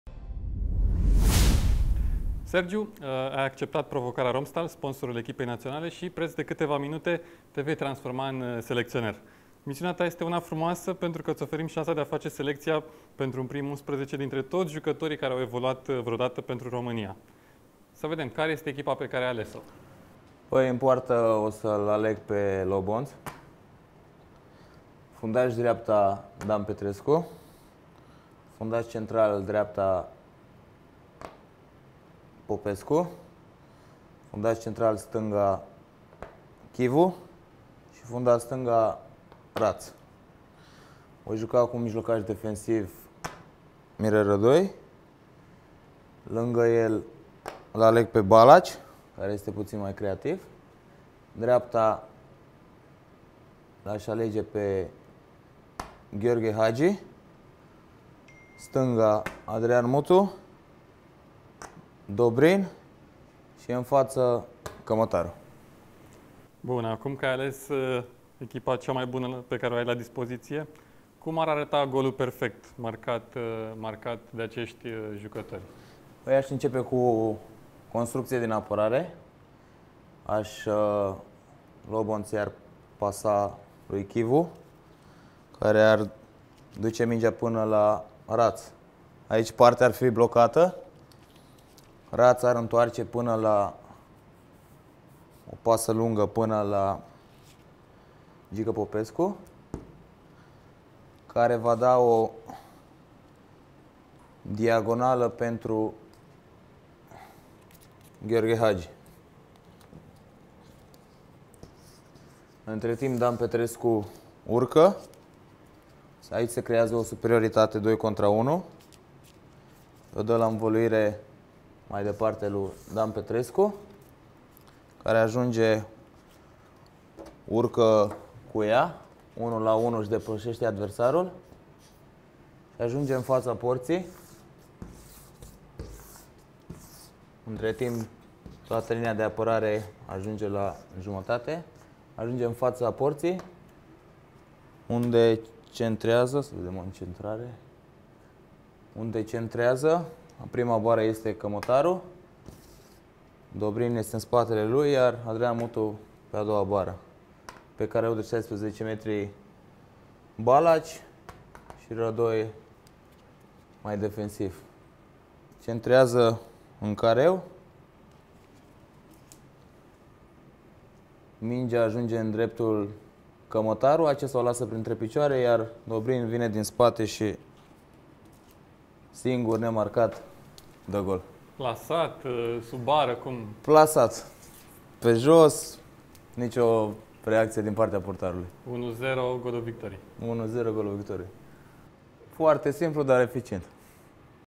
Echipa de vis a ”selecționerului” Hanca
Deși are doar 28 de ani, tricolorul Sergiu Hanca s-a pliat perfect pe rolul de selecționer, cel puțin câteva minute! Asta pentru că a acceptat provocarea Romstal – Sponsor al Echipei Naționale și a alcătuit echipa ideală all-time a Românei. Apoi, ne-a arătat ce gol minunat marchează aceasta, începând de la portarul Lobonț, pentru victoria cu 1-0! Urmărește-ne si pe: